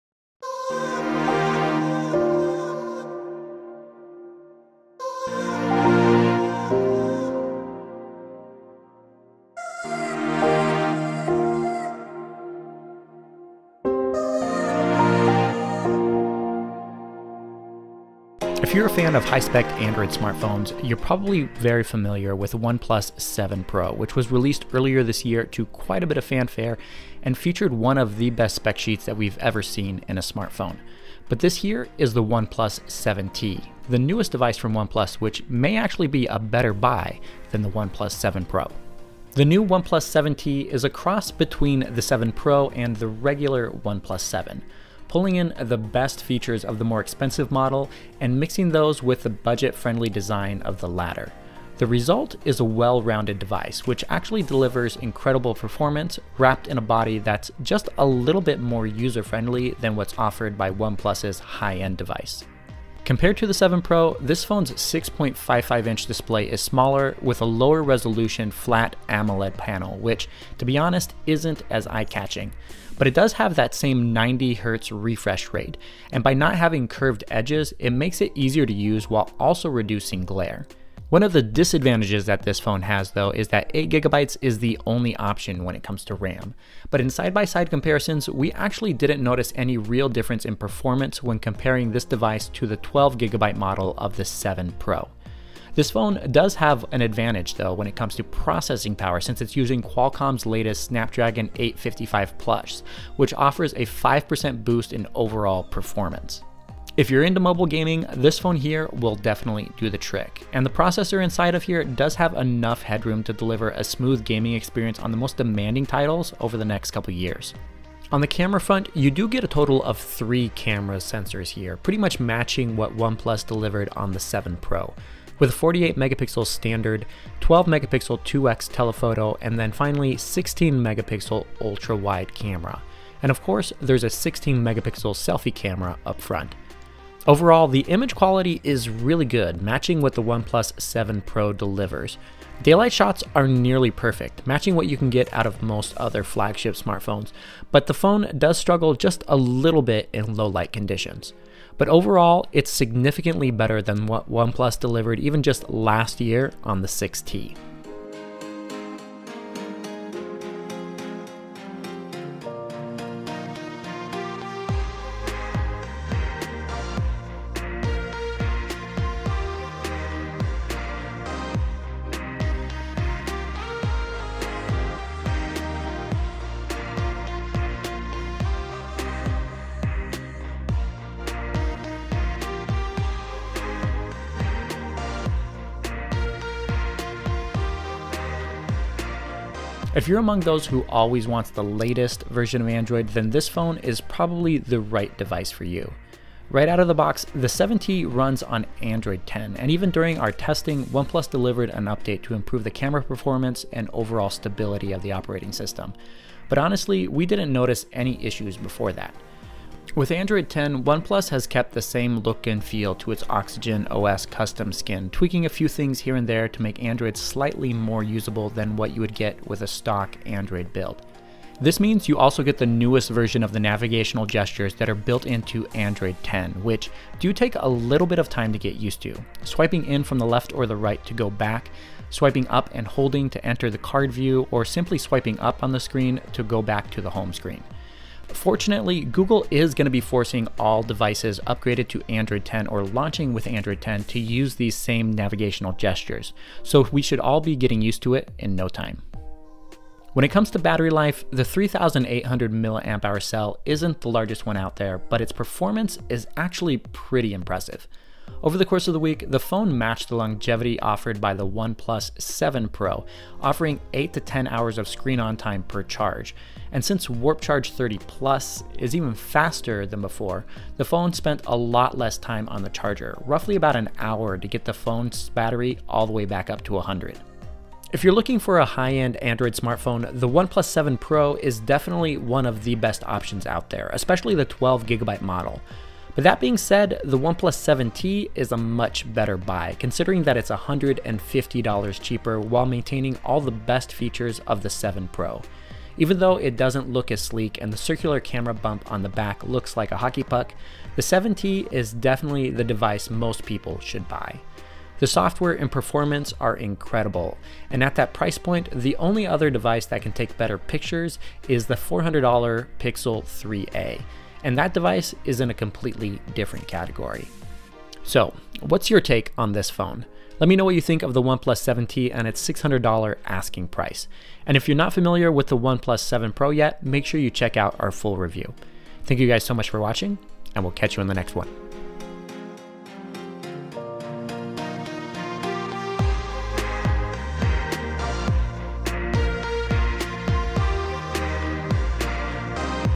OnePlus 7T review: going pro on a budget
The OnePlus 7T is coming this month and our review of the latest OnePlus phone has finally landed. In our OnePlus 7T review we take a look at how the phone differs from the OnePlus 7 Pro and which device users should buy.

For our written OnePlus 7T review, be sure to check out